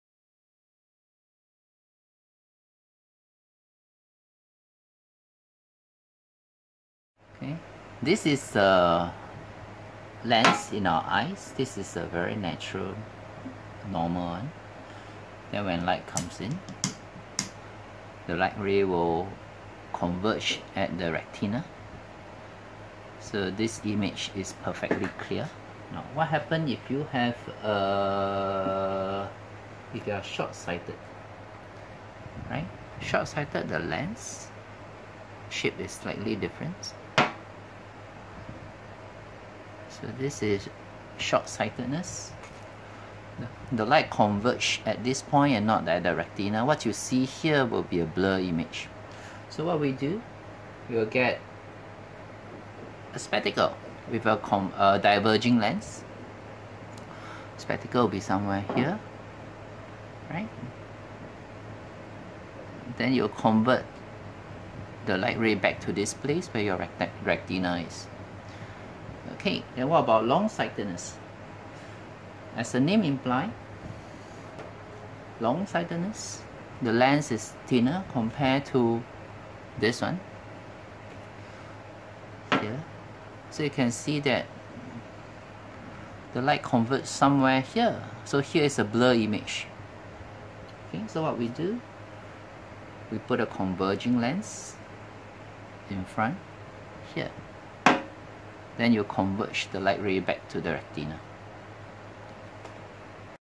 long sightedness and short sightedness demo
long sightedness and short sightedness demo demonstrates how to use diverging and converging lens to correct ong sightedness and short sightedness respectively.